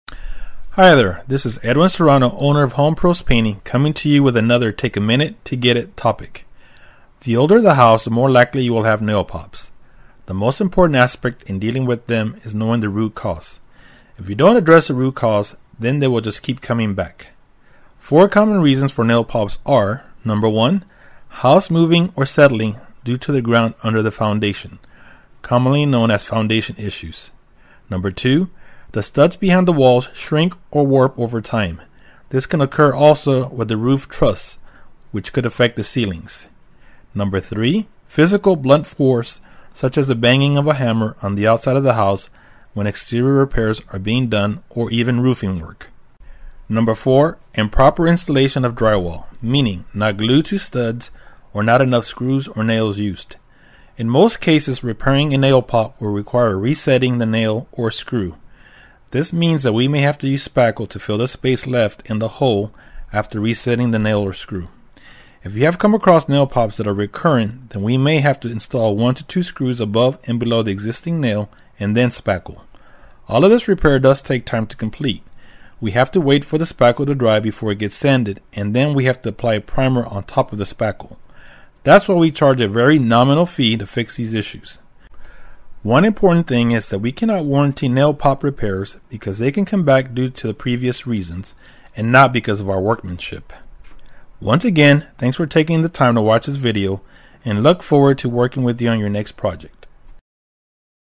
Interior Painting Process: Fixing Nail Pops San Antonio, Texas
Shrinking wood  such as studs or roof truss movement for ceilings.
House movement caused be foundation issues or settling.
Physical force such banging of a hammer or walking in attic.
Improper  installation of drywall—not glued; not enough screw/nails.

In most instances repairing a nail pop will require re-setting the nail or screw.  This means that we may have to use spackle to fill the space left in
the hole after re-setting the nail/screw.  If you have come across nail pops that are recuring then we may have to install 1 to 2 screws above and
below the existing nail and then spackle.  All of this repair does take time to complete.  Then we have to wait for the spackle to dry before it get
sanded and then we have to apply primer on top of the spackle.

We cannot warranty nail pop repairs because they can come back due to the
above reasons which are out of our control.